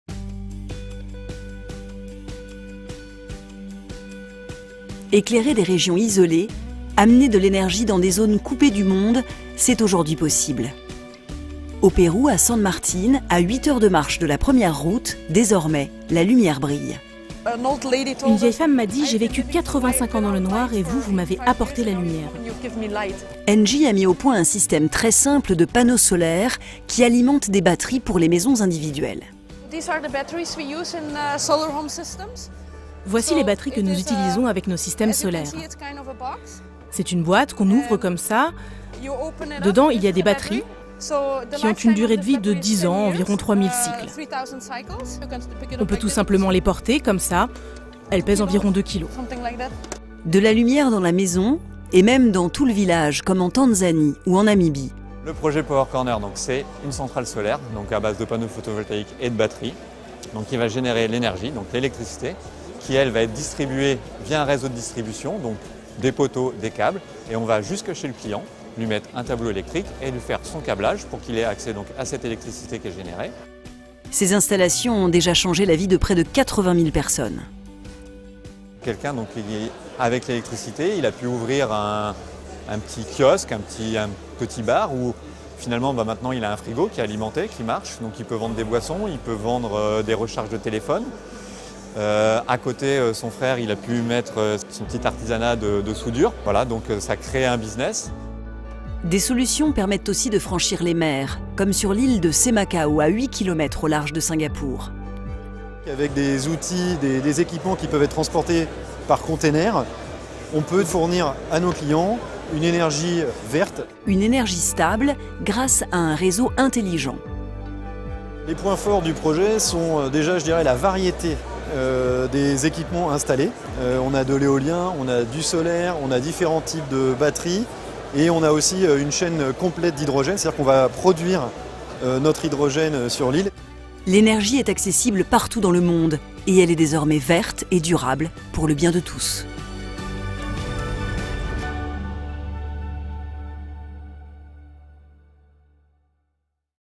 L'accès à l'énergie par ENGIE
Dans le monde entier, ENGIE donne accès à l'énergie aux populations grâce aux énergies renouvelables.